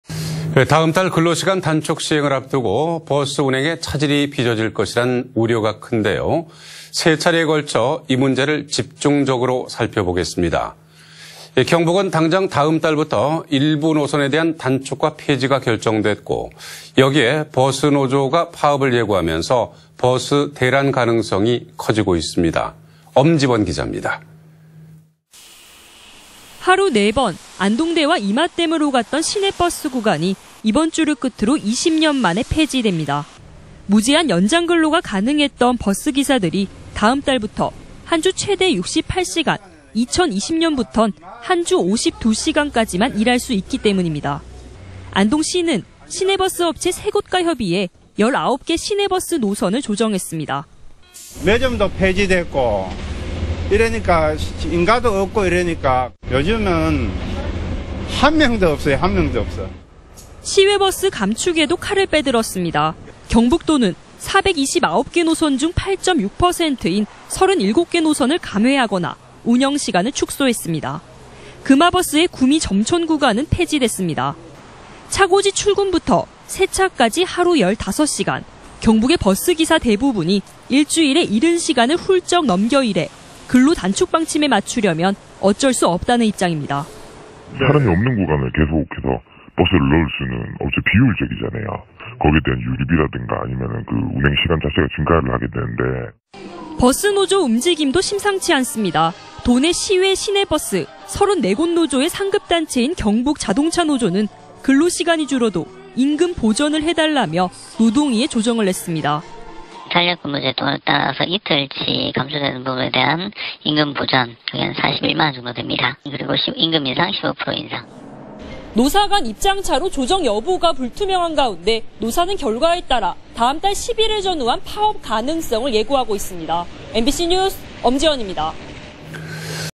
[안동MBC뉴스]노선 감축에 파업 예고.."위기의 경북버스"
2018/06/26 11:28:15    작성자 : 엄지원

◀ＡＮＣ▶
다음달 근로시간 단축 시행을 앞두고
버스운행에 차질이 빚어질 것이라는 우려가
큰데요, 3차례에 걸쳐 이 문제를
집중적으로 살펴보겠습니다.

경북은 당장 다음달부터 일부 노선에 대한
단축과 폐지가 결정됐고 여기에 버스노조가
파업을 예고하면서 버스대란 가능성이 커지고 있습니다.

엄지원 기자
◀ＥＮＤ▶
◀ＶＣＲ▶

하루 네 번
안동대와 임하댐을 오갔던 시내버스 구간이,
이번주를 끝으로 20년 만에 폐지됩니다.

무제한 연장 근로가 가능했던 버스기사들이
다음달부터 한주 최대 68시간, 2020년부턴
한주 52시간까지만 일할 수 있기 때문입니다.

안동시는 시내버스 업체 3곳과 협의해
19개 시내버스 노선을 조정했습니다.

◀ＩＮＴ▶박주동/11번 버스기사
(임하댐) 매점도 폐지됐고 민가도 없고 그러니까 요즘은 (승객이) 한명도 없어요, 한명도 없어..

시외버스 감축에도 칼을 빼들었습니다.

경북도는 429개 노선 중 8.6%인 37개 노선을
감회하거나 운영시간을 축소했습니다.

금아버스의 구미-점촌 구간은 폐지됐습니다.

차고지 출근부터 세차까지 하루 15시간,
경북의 버스기사 대부분이 일주일에 70시간을 훌쩍 넘기면서, 근로 단축방침에 맞추려면
어쩔 수 없다는 입장입니다.

◀ＳＹＮ▶교통행정과 공무원
사람이 없는 구간을 계속해서 버스를 넣을 수는 없어요, 비효율적이죠. 거기에 대한 유류비라든가 운행시간 자체가 증가하게 되는데..

버스노조 움직임도 심상치 않습니다.

도내 시외.시내버스 34곳 노조의 상급단체인
경북 자동차노조는 근로시간이 줄어도
임금보전을 해달라며 노동위 조정을 냈습니다.

◀ＳＹＮ▶경북노동위원회 관계자
(노조안은) 탄력근무제 도입에 따라 이틀치 감소의 임금보전 41만원 정도, 임금인상 15%..

(S/U) 노사간 입장차로 조정여부가 불투명한 가운데 노조는 결과에 따라 다음달 10일을
전후한 파업 가능성을 열어두고 있습니다.